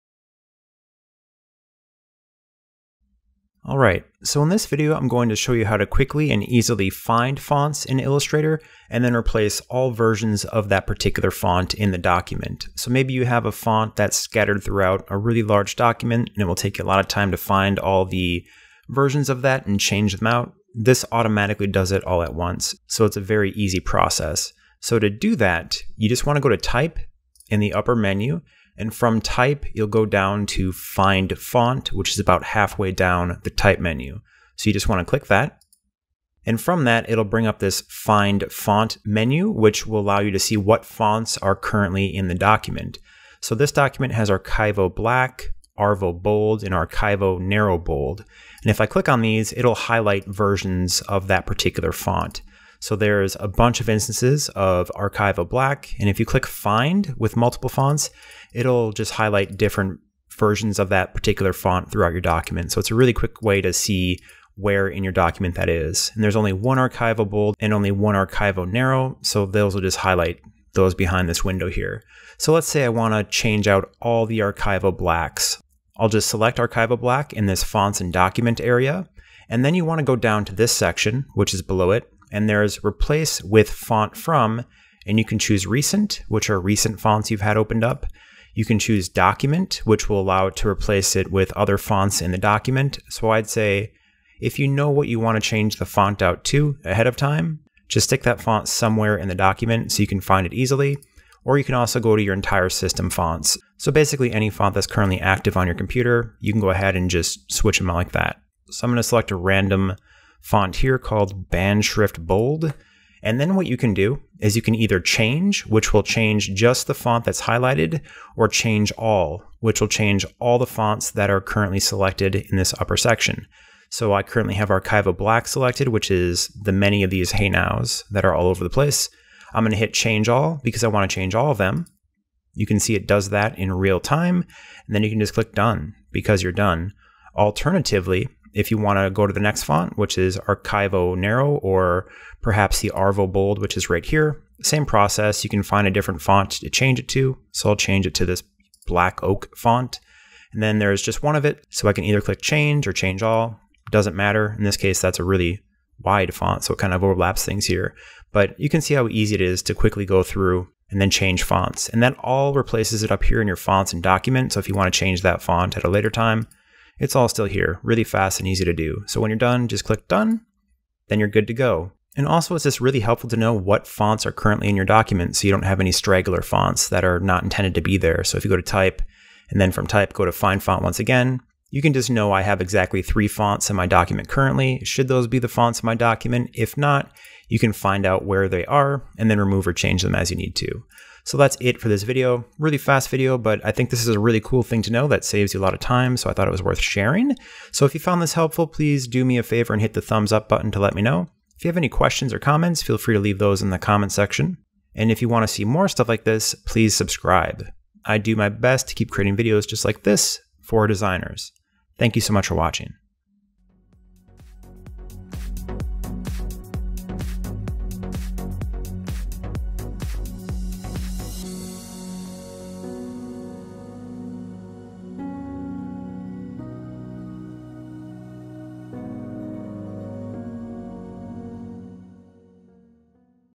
How to Quickly Find and Change Multiple Fonts at Once in Adobe Illustrator Tutorial
Learn a simple trick to quickly find and change fonts inside Adobe Illustrator

This Adobe Illustrator tutorial goes over how you can quickly and easily find and change multiple fonts at once! Sometimes you have a font scattered across a document and you need to change every instance of it at once. This tip lets you do that quickly and easily all at once, saving the need to find and change them individually. You can decide if you want to change fonts one by one, or swap them all across the document with a single click.

Have any questions about what you've seen in the video?